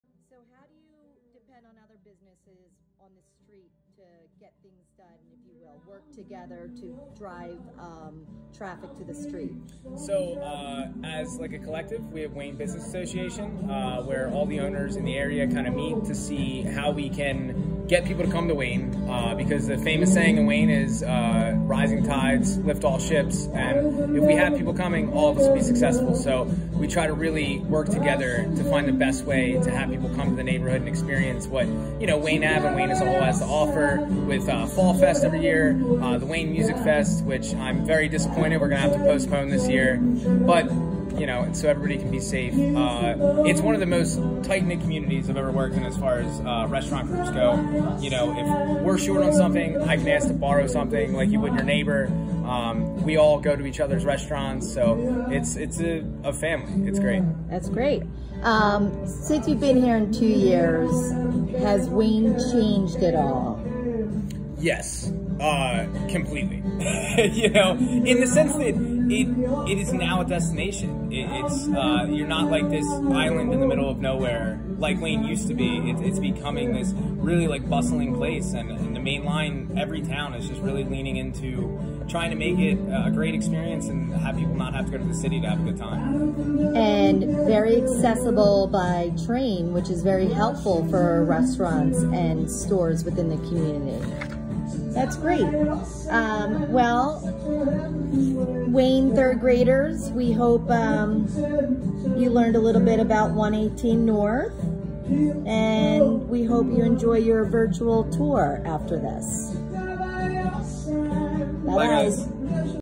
Visit 118 North a Restaurant, Bar and Live Music Venue 2 of 2
Meet General Manager Mike Shane and learn about the goods and services 118 North provides to their customers.